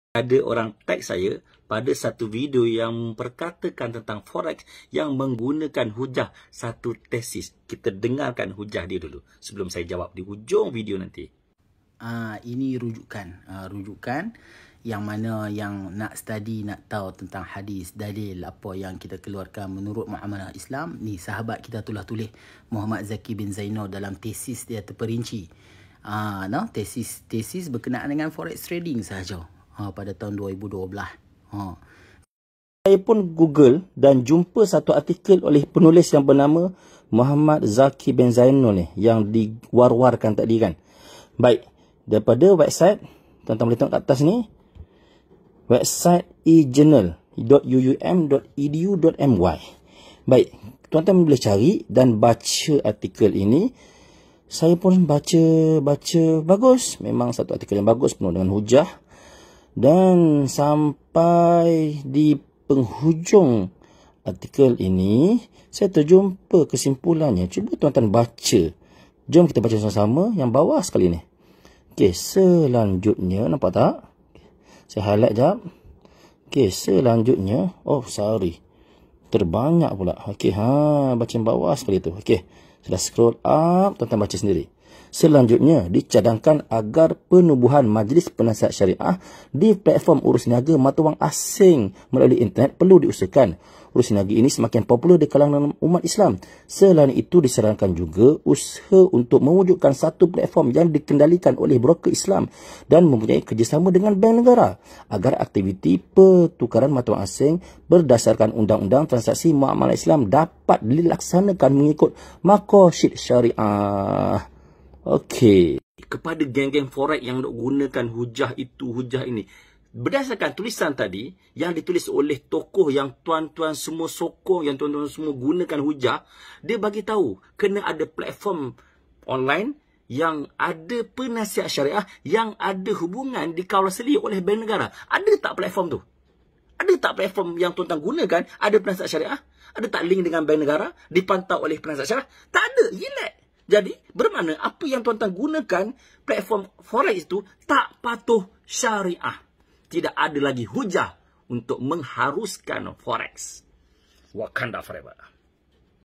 Menjawab Hujah Geng Forex - Part 1
Menjawab hujah geng forex. Mereka guna hujah satu tesis yang dikatakan di dalamnya mengharuskan forex. Tapi apabila kita baca.